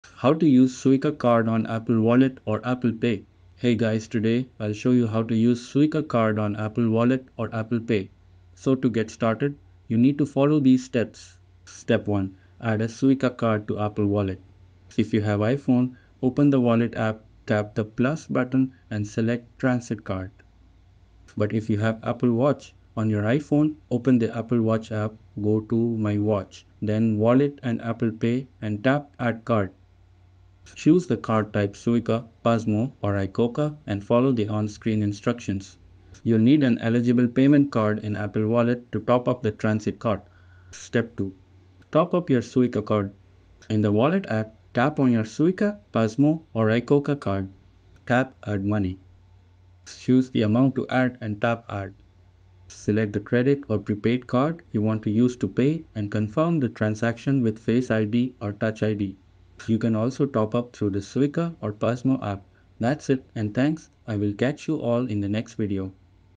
How to Use Suica Card on Apple Wallet & Apple Pay – Complete Guide 2025
In this comprehensive tutorial, learn how to seamlessly add your Suica Card to Apple Wallet and use it with Apple Pay in 2025. Follow these easy steps:
1. Open the Wallet app on your iPhone.
2. Tap the "+" icon to add a new card.
3. Select "Transit Card" and choose Suica from the options.
4. Follow the prompts to scan or enter your Suica Card details.
5. Once added, top up your balance directly through Apple Pay.
6. Use your iPhone for quick and easy payments at transit gates and participating retailers.
Unlock the convenience of digital payments with your Suica Card today!